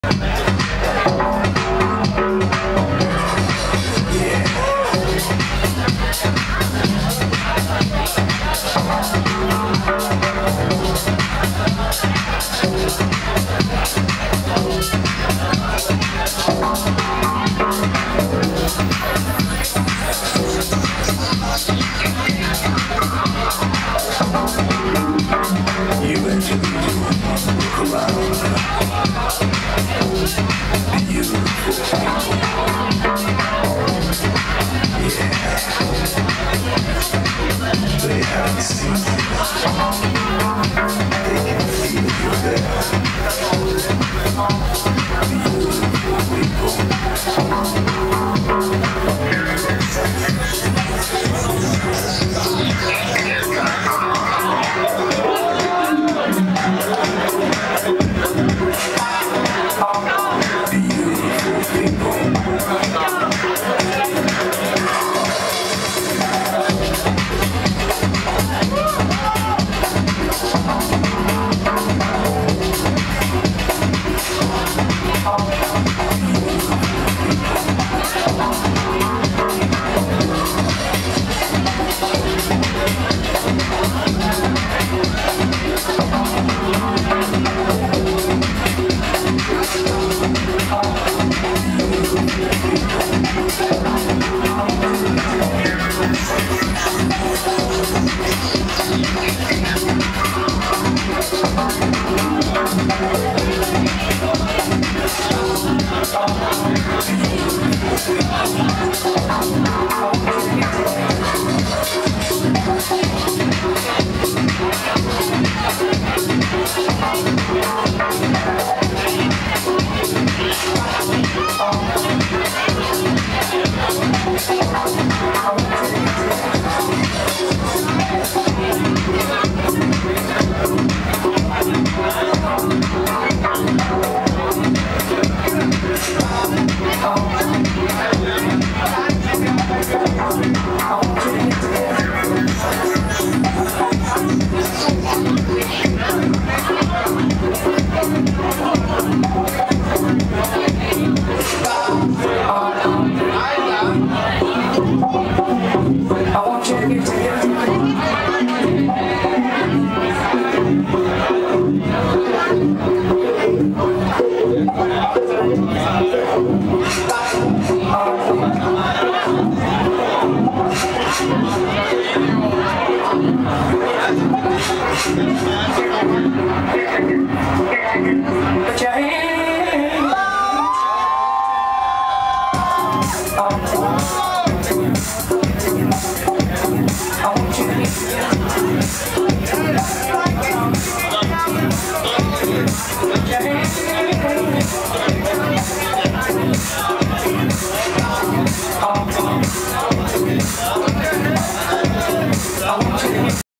Claudio Ricci @ 5 Years Club Camouflage Part 2 Sept 2009
Awesome 5 year anniversary with Frank Lorbeer (Cocoon)